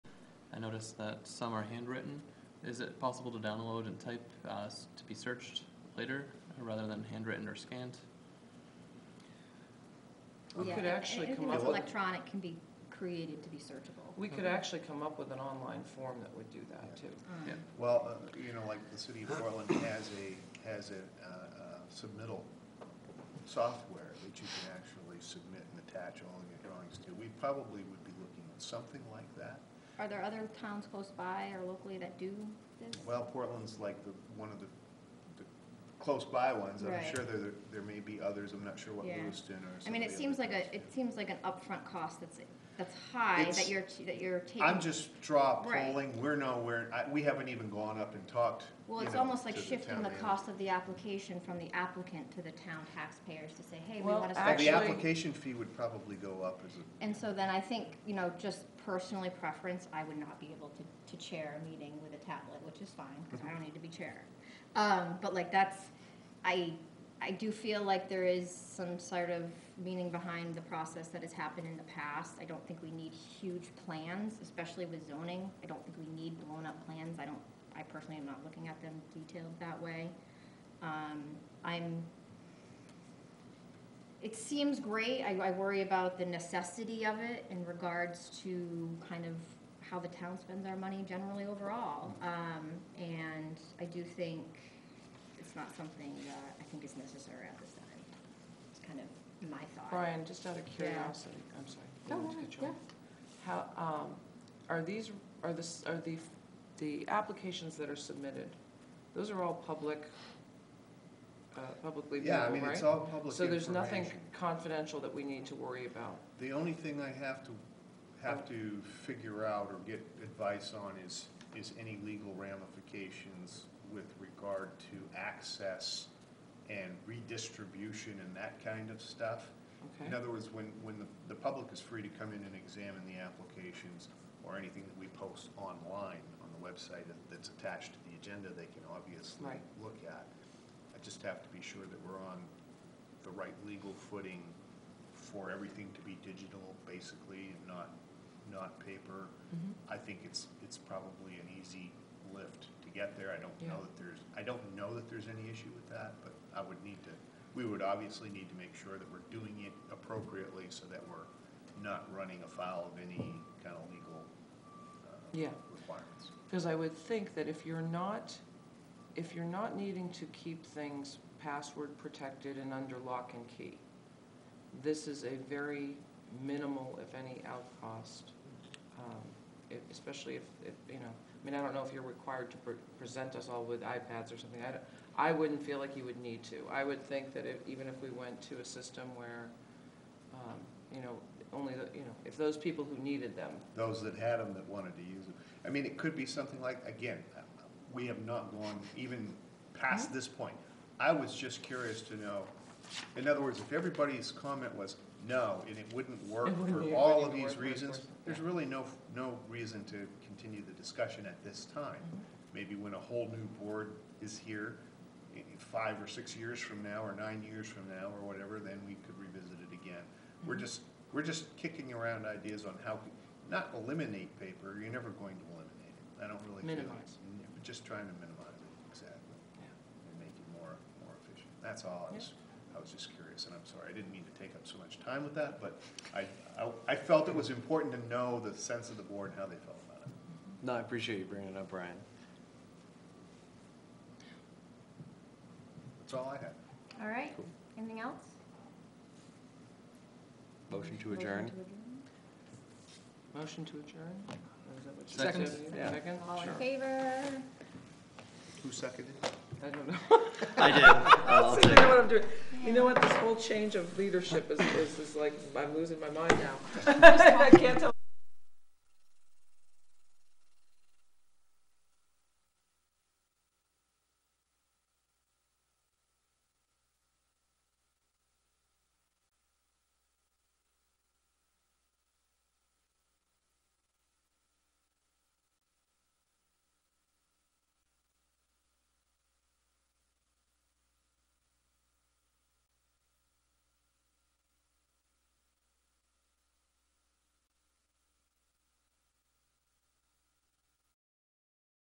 Zoning Board, Feb. 13, 2019, Scarborough, Maine
Zoning Board, Feb. 13, 2019, Scarborough, Maine meeting. 5 minutes. Due to Camera Operator Error this meeting was not recorded from the beginning, there are approximately 10 to 15 minutes missing.